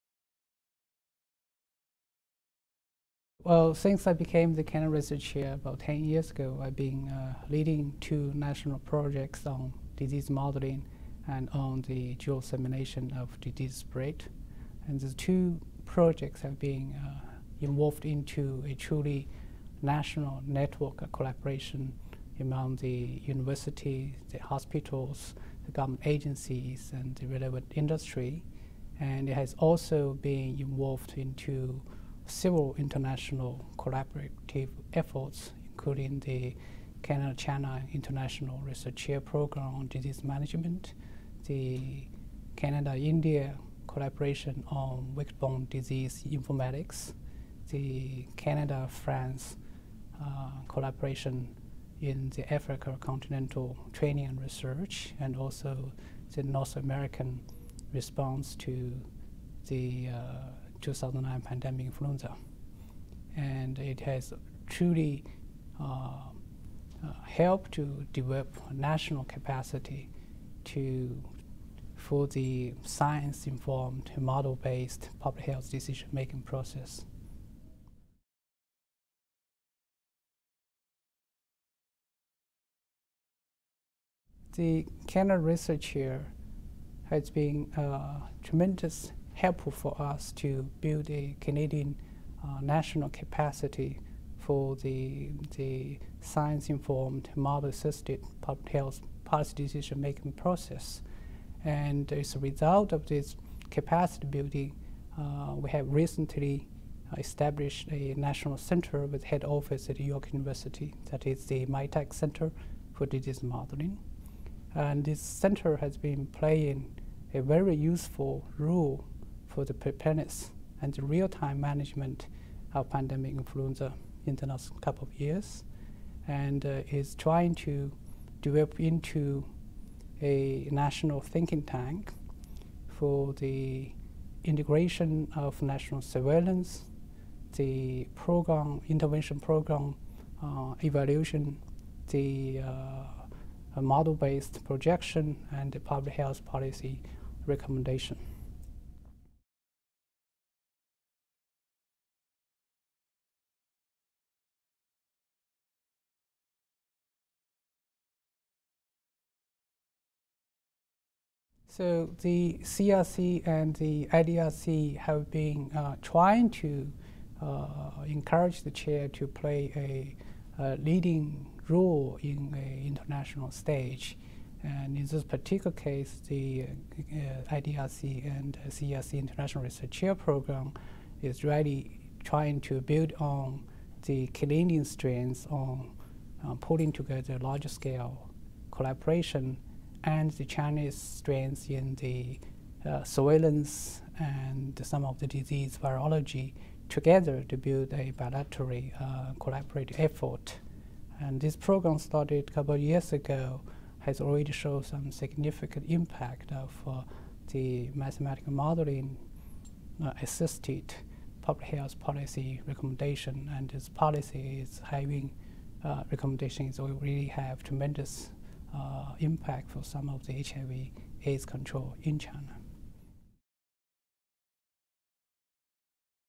Jianhong Wu, mathematician and Canada Research Chair
York Professor Jianhong Wu, Canada Research Chair in Industrial and Applied Mathematics, talks about his work and being a CRC.